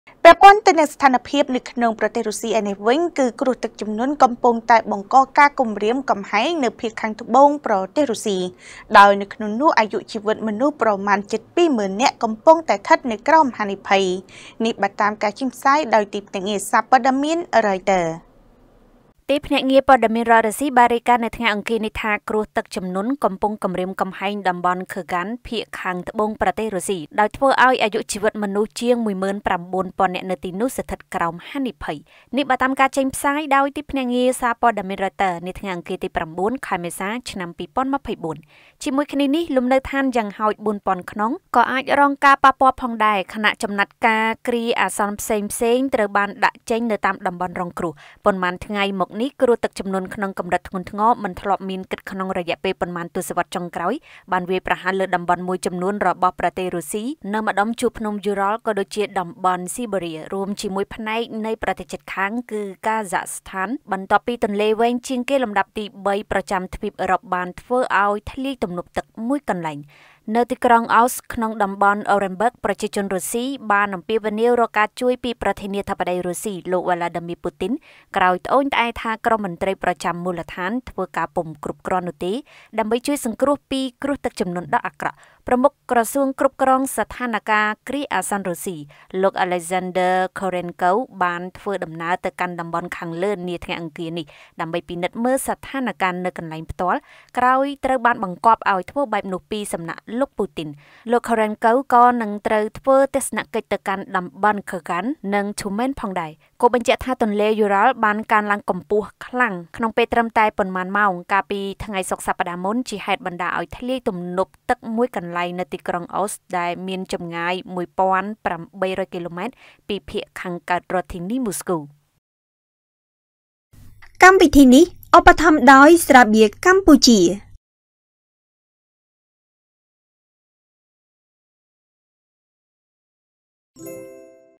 គ្រោះទឹកជំនន់នៅភាគខាងត្បូងប្រទេសរុស្ស៉ី កំពុងគំរាមកំហែងអាយុជីវិតមនុស្សជិត២០,០០០នាក់
* រាយការណ៍ដោយកញ្ញា ថាន  រស្មី និងកញ្ញា យឹម ចាន់ស្រីវីន